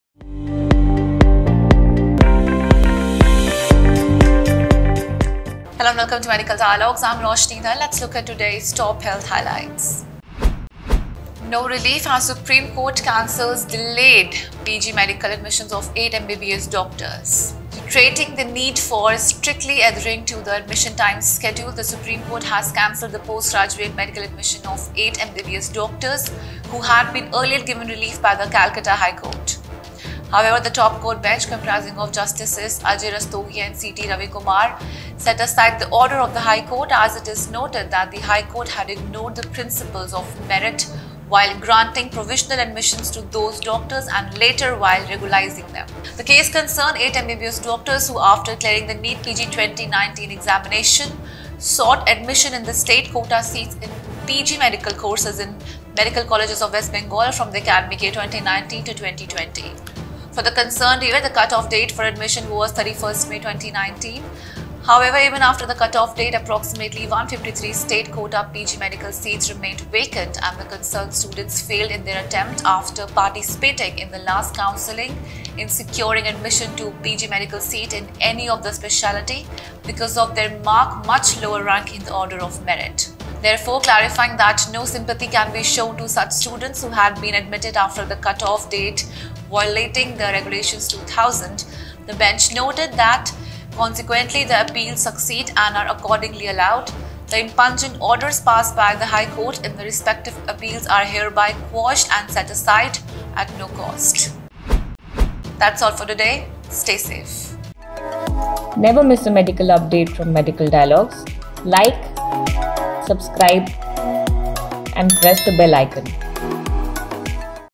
NO Relief, Supreme Court Cancels Delayed PG Medical Admissions Of 8 MBBS Doctors, Says Schedule Must
Reiterating the need for strictly adhering to the admission time schedule, the
SC has cancelled the Postgraduate medical admission of eight MBBS doctors, who had been earlier given 4    relief by the Calcutta High Court.